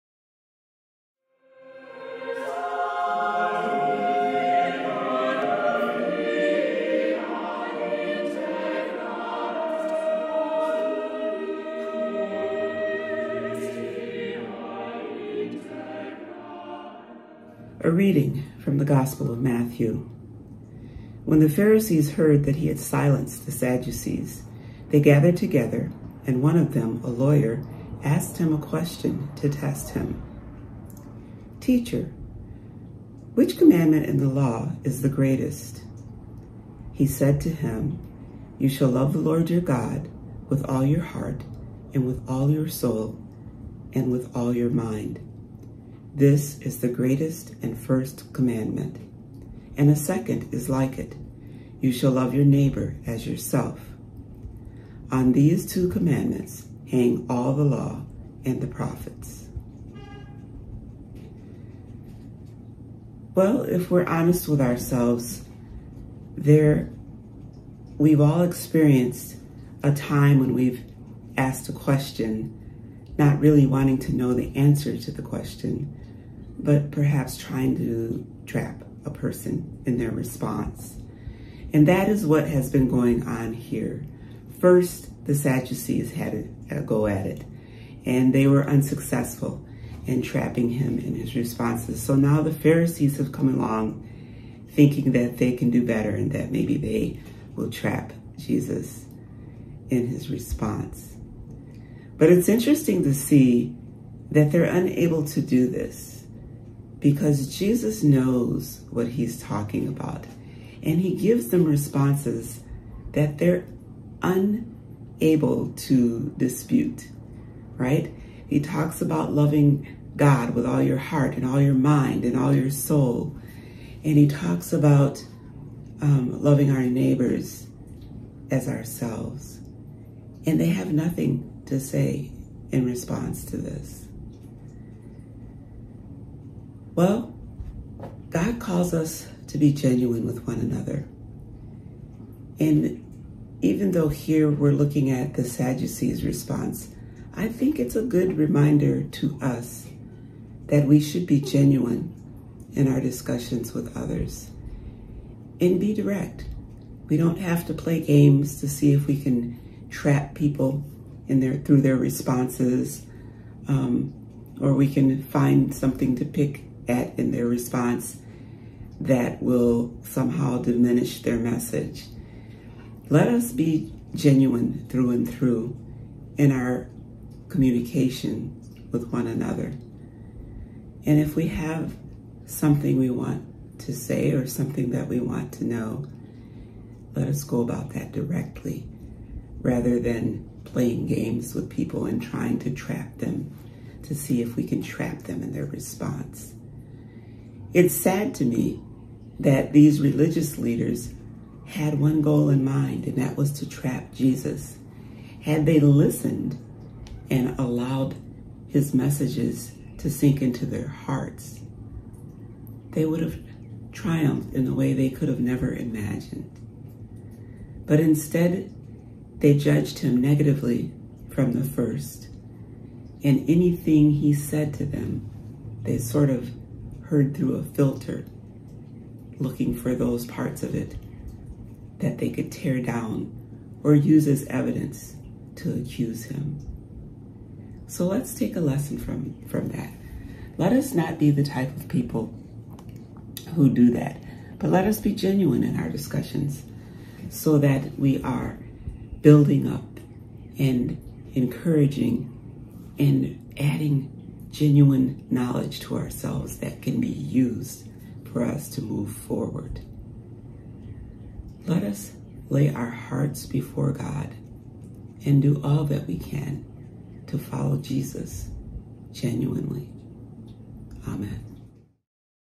Don't Play Games
In today's SermonOfTheDay, the Rev. Brenda Kilpatrick reminds us that Jesus knows what he's talking about, and that we should learn from him; be genuine and direct in our conversations with one another and don't play games.